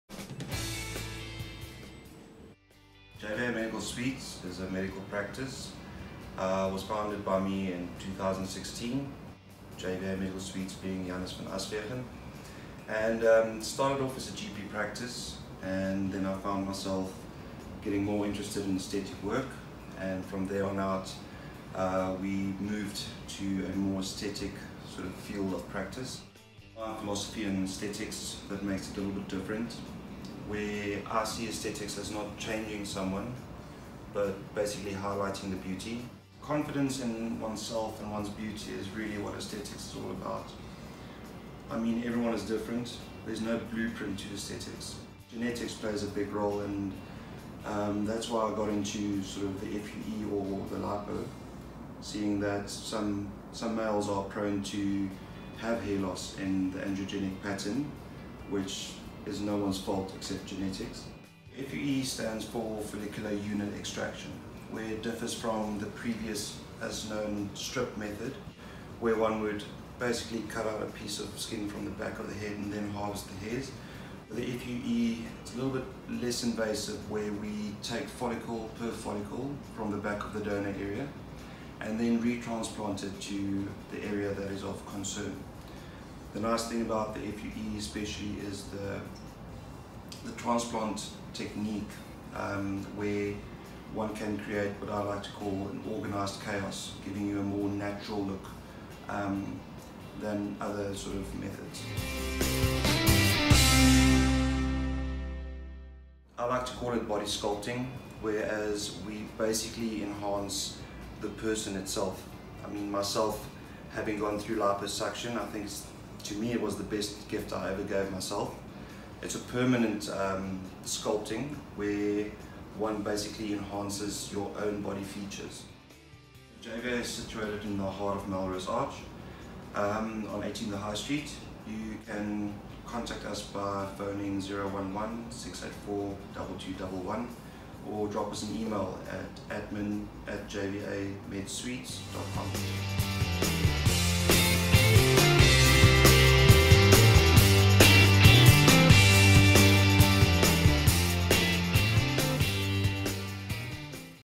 JVA Medical Suites - DR JANNES VAN ASWEGEN BIO
Dr Van Aswegen (GPSA) from South-Africa, one of the leading Aesthetic GP's in Sandton and the greater JHB, with experience in Vaser™ Lipo, FUE™ Hair Transplant and more.  See the video for more information!